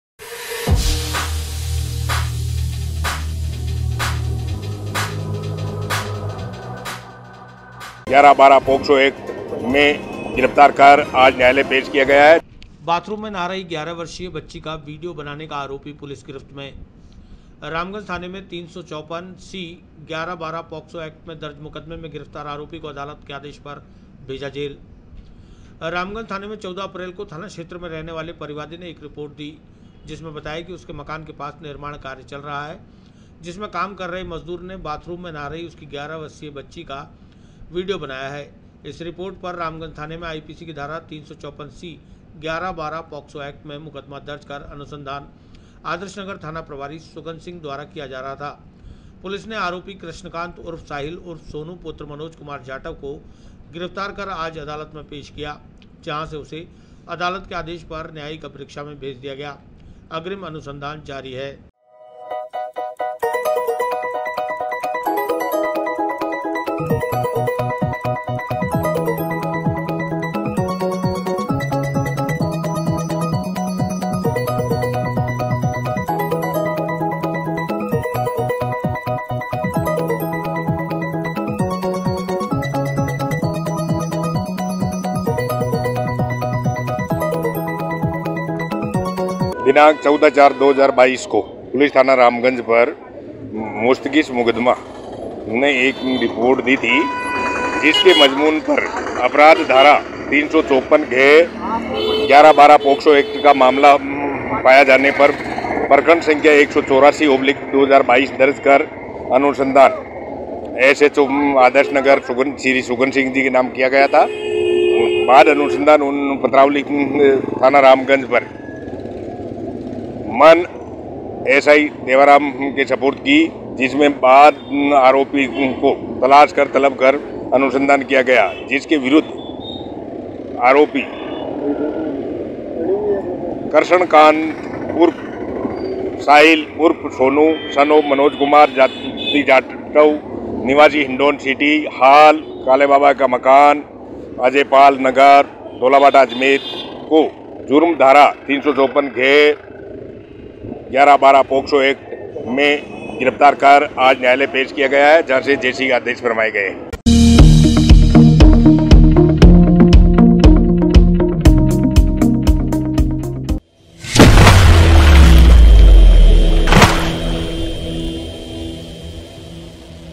बाथरूम में नहा रही 11 वर्षीय बच्ची का वीडियो बनाने का आरोपी पुलिस गिरफ्त में
रामगंज थाने में 354 11,12 पॉक्सो एक्ट में दर्ज मुकदमें में गिरफ्तार आरोपी को अदालत के आदेश पर भेजा जेल
रामगंज थाने में 14 अप्रैल को थाना क्षेत्र में रहने वाले परिवादी ने एक रिपोर्ट दी जिसमें बताया कि उसके मकान के पास निर्माण कार्य चल रहा है जिसमें काम कर रहे मजदूर ने बाथरूम में नहा रही उसकी 11बच्ची का वीडियो बनाया है। इस रिपोर्ट पर रामगंज थाने में आईपीसी की धारा 354 सी 11- 12 पोक्सो एक्ट में मुकदमा दर्ज कर अनुसंधान आदर्श नगर थाना प्रभारी सुगन सिंह द्वारा किया जा रहा था। पुलिस ने आरोपी कृष्णकांत उर्फ साहिल उर्फ सोनू पुत्र मनोज कुमार जाटव को गिरफ्तार कर आज अदालत में पेश किया जहां से उसे अदालत के आदेश पर न्यायिक अभिरक्षा में भेज दिया गया अग्रिम अनुसंधान जारी है।